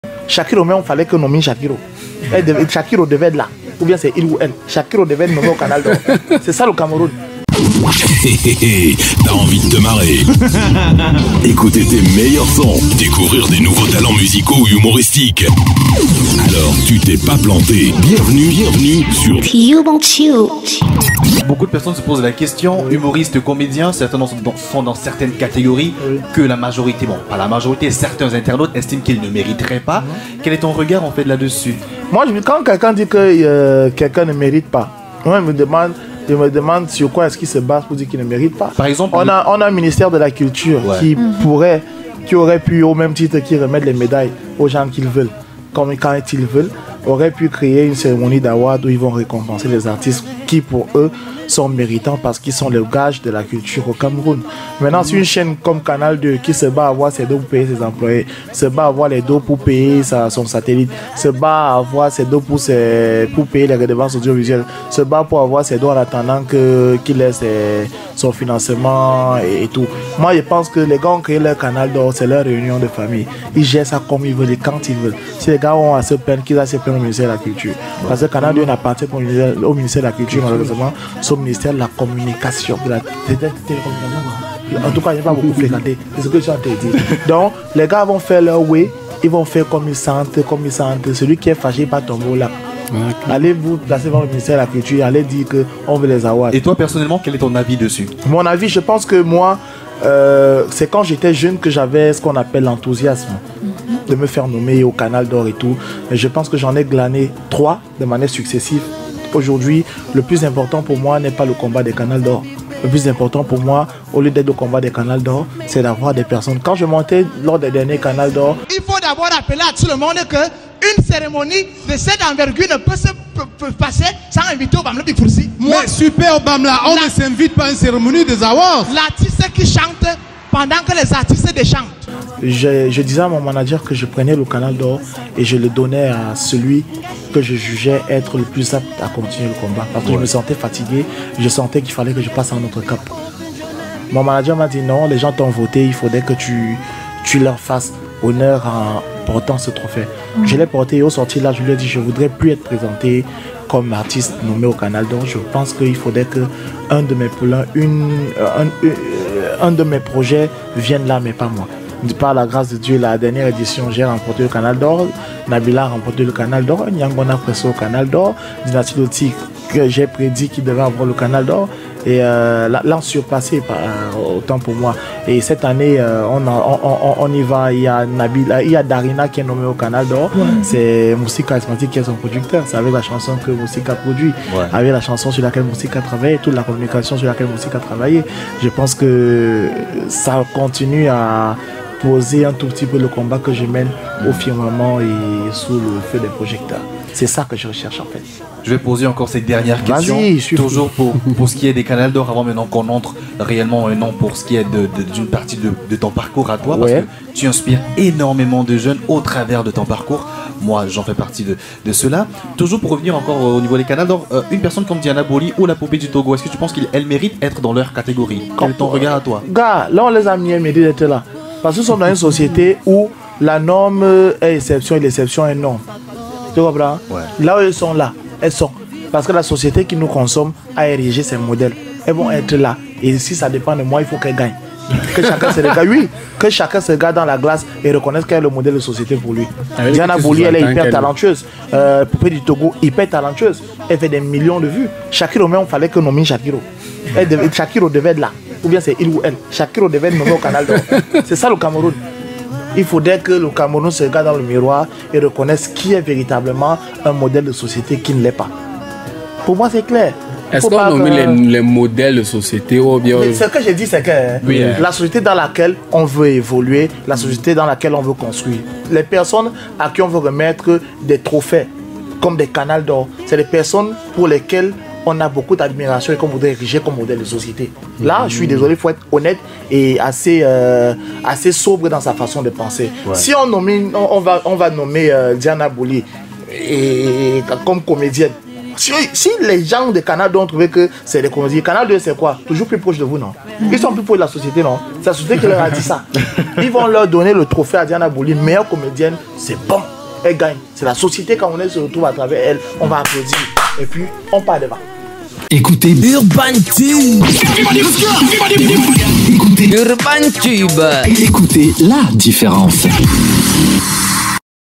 Moustik le Karismatik parle des Canal d’or , la poupée du Togo et Diana Bouli
Équipe technique
Mise en onde: Franck Narcisse
Réalisation : Arsène Nguela
Présentation : Servais Noudou, Orneila reine, Nicole Nde, Roxane Audrey Ngo & Luxandra Wandji
Producteur : Servais Noudou
Diffuseur : i FM